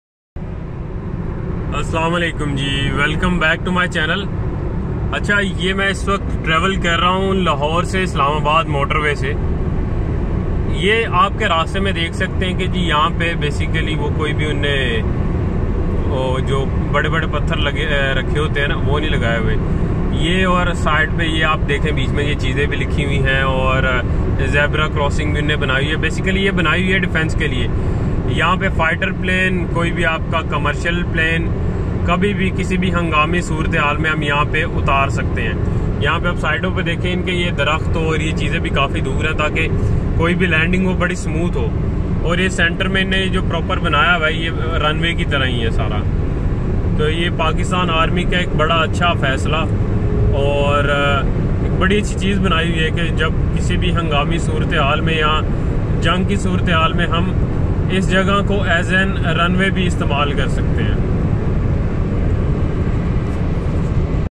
Lahore Islamabad motorway amazing work by Pakistan
The best and fast way to go anywhere is motorways and for best business opportunities.